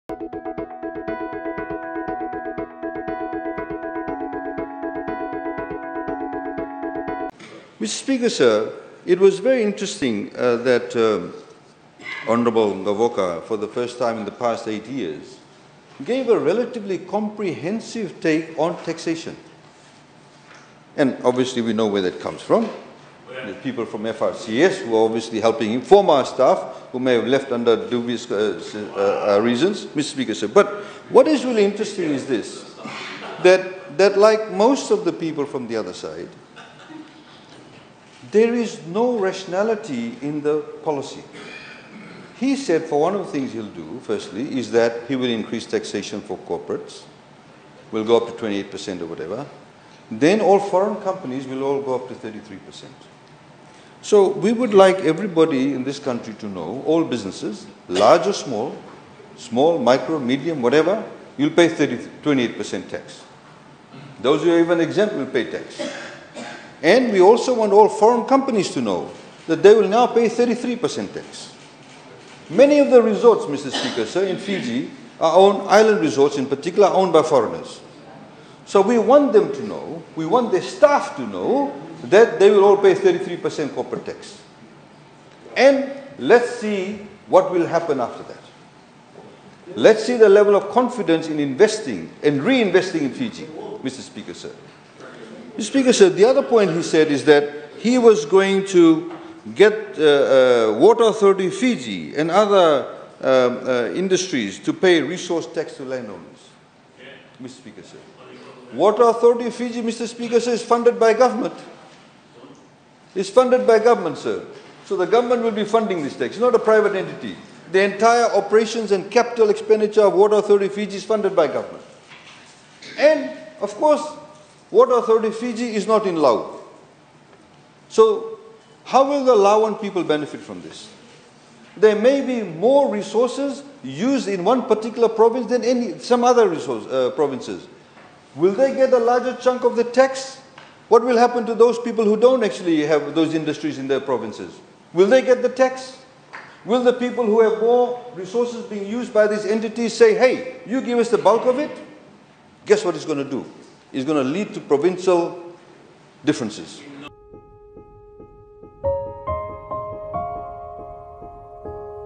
FijiFirst | Fijian Government | 2022-2023 Budget Debate | Hon. Attorney-General
"It was very interesting that Hon. Gavoka, for the first time in the past eight years, gave a relatively comprehensive take on taxation. And obviously, we know where that comes from. The people from FRCS were obviously helping him from our staff who may have left under dubious reasons. But what is really interesting is this, that like most of the people from the other side, there is no rationality in the policy."

- Hon. Aiyaz Sayed-Khaiyum, Attorney-General and Minister for Economy, Civil Service, Communications, Housing and Community Development